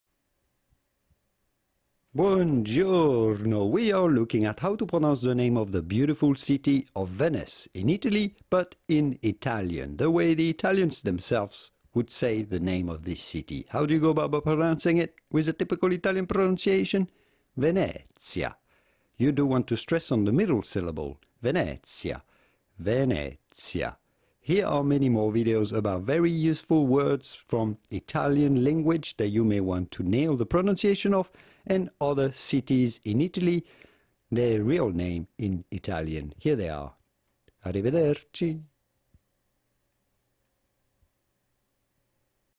How to say Venice in Italian? How to Pronounce Venezia?
Listen and learn how to say Venezia correctly (city of Venice in Italiano language, original name of the city) with Julien, "how do you pronounce" free pronunciation audio/video tutorials.

About Venezie/Venice: Venice, the capital of northern Italy’s Veneto region, is built on more than 100 small islands in a lagoon in the Adriatic Sea. It has no roads, just canals – including the Grand Canal thoroughfare – lined with Renaissance and Gothic palaces. The central square, Piazza San Marco, contains St. Mark’s Basilica, which is tiled with Byzantine mosaics, and the Campanile bell tower offering views of the city’s red roofs. Source